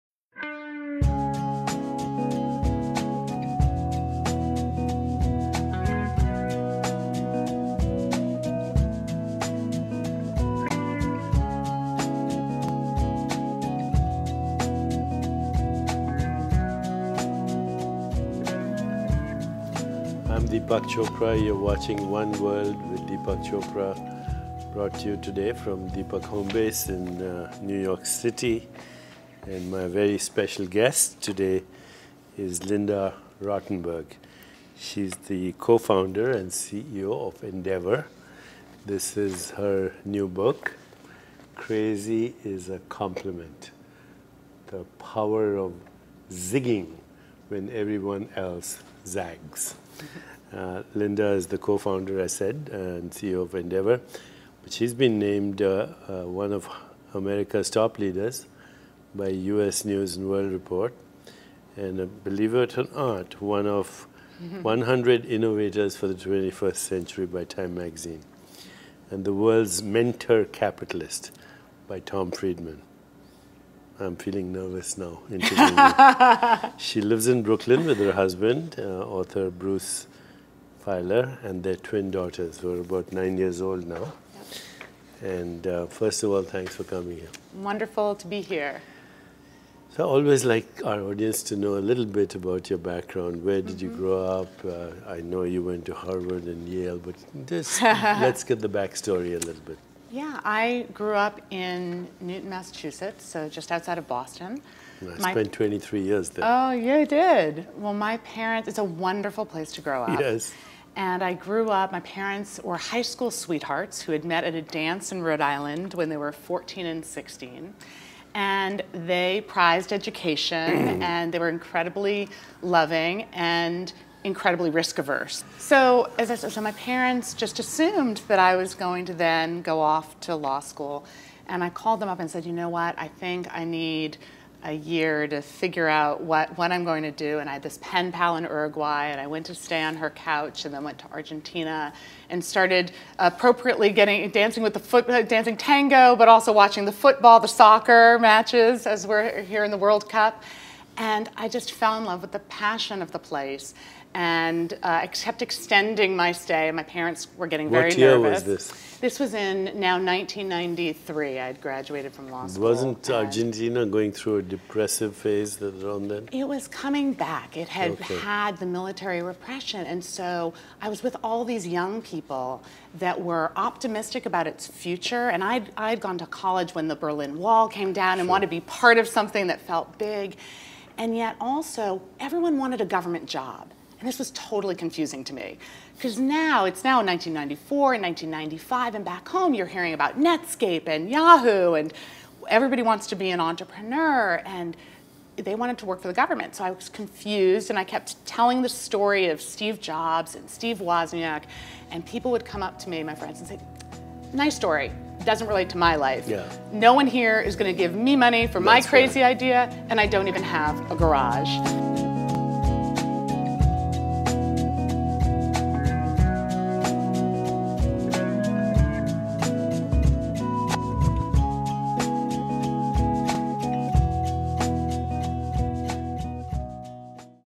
ONE WORLD: LINDA ROTTENBERG & DEEPAK CHOPRA
Author of CrazyisaCompliment, LindaRottenberg & her organization Endeavor have supported 600+ companies across 20 countries, generating 400,000 jobs, with over $7 billion in revenues.